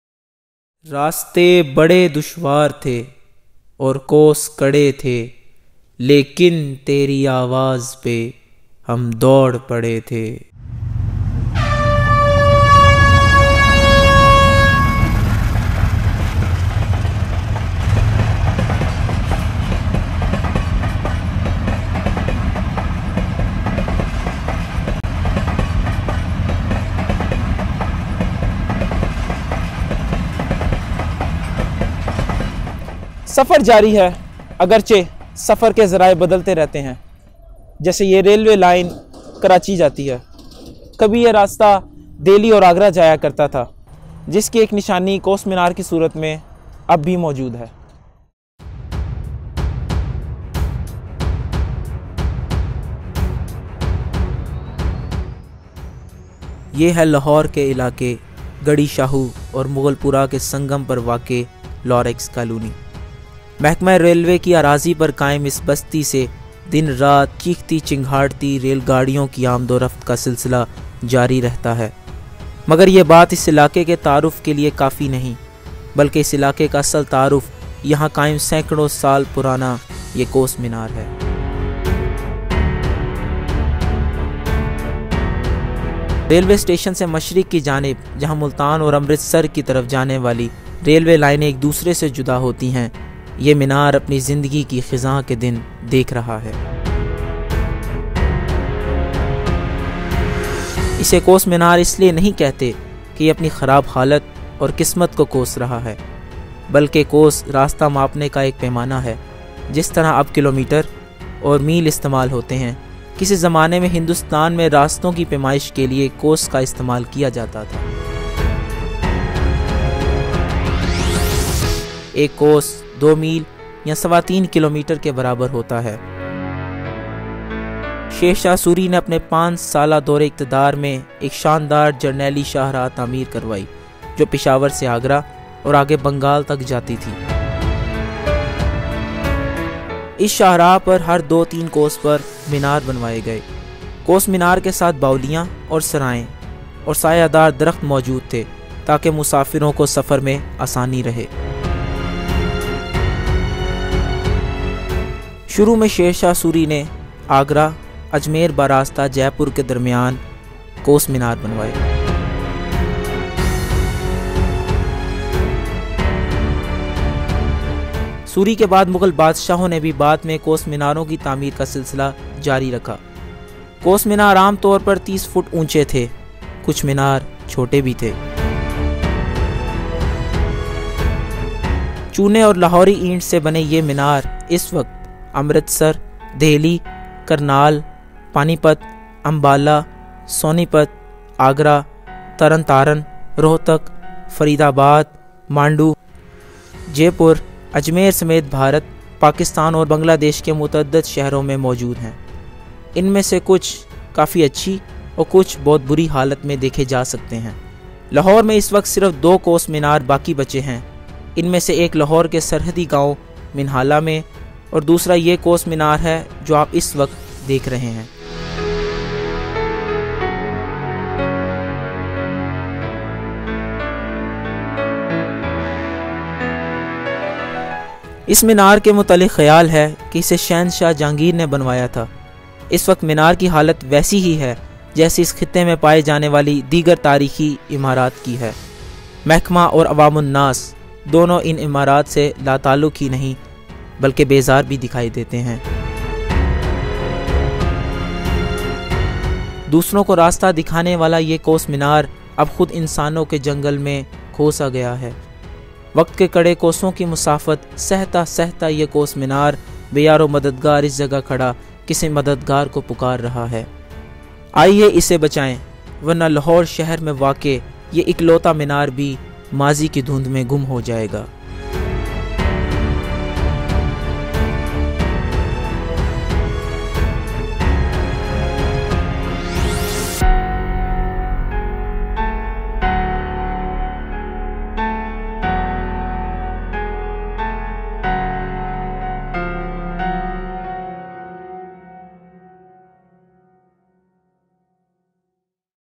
Kos Minar Lahore | The only Surviving Mile Pillar in Lahore City
The Kos Minars (Mile Pillars) are medieval Indian milestones along the Grand Trunk Road in northern Indian subcontinent, that were introduced by the 16th-century Pashtun ruler Sher Shah Suri. Kos Minars were erected to serve as markers of distance along royal routes from Agra to Ajmer, Agra to Lahore, and from Agra to Mandu in the south.